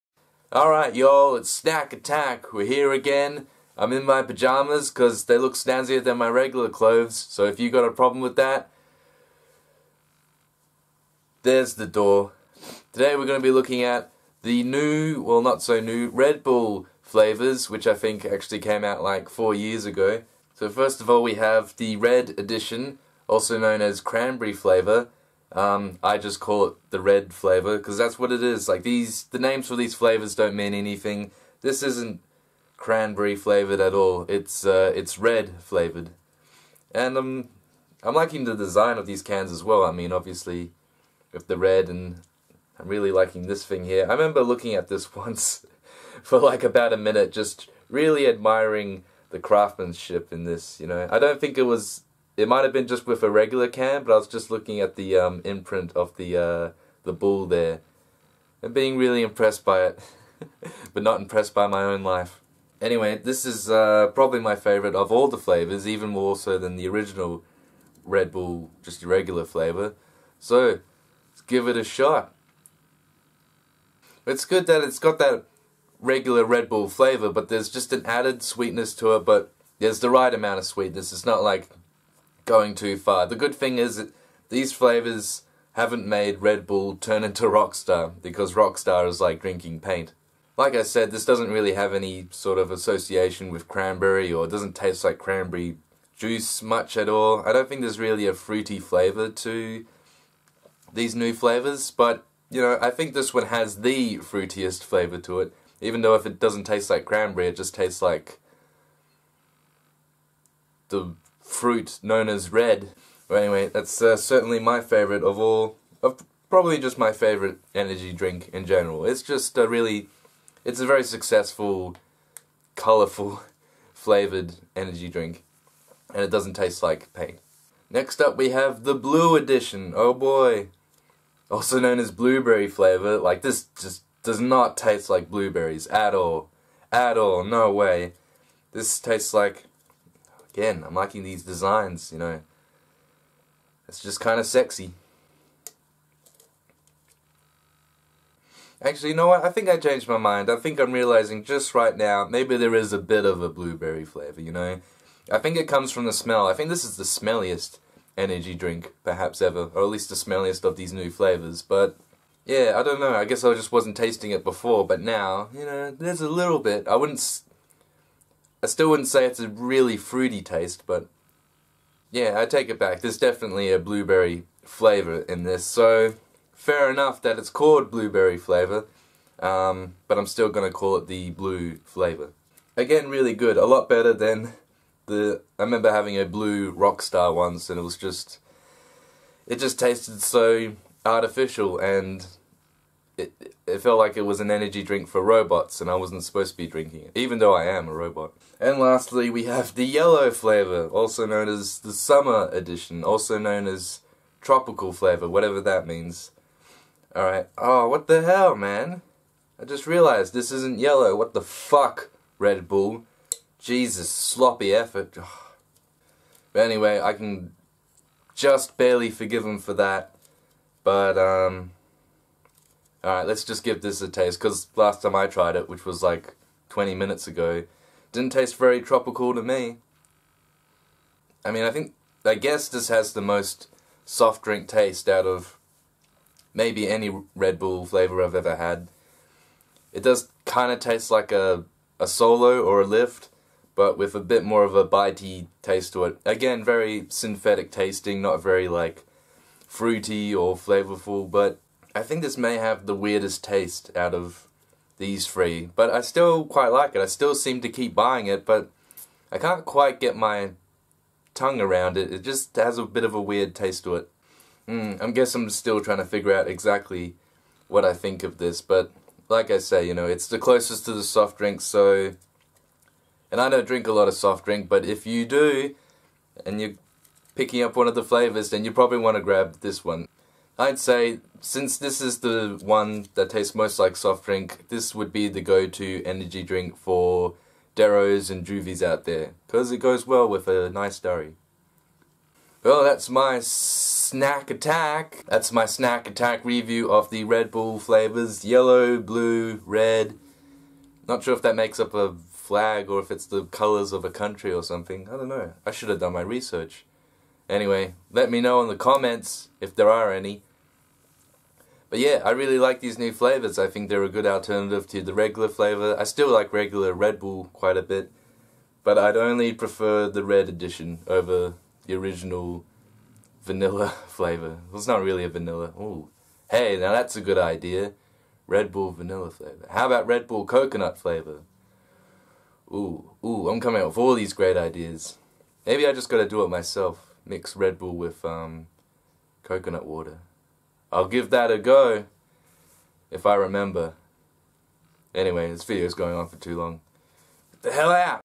Red Bull Cranberry, Blueberry, and Tropical flavours review- SNACK ATTACK!!
Reviewing the three alternative flavours Red Bull brought out for its own cramp-inducing gift to the world.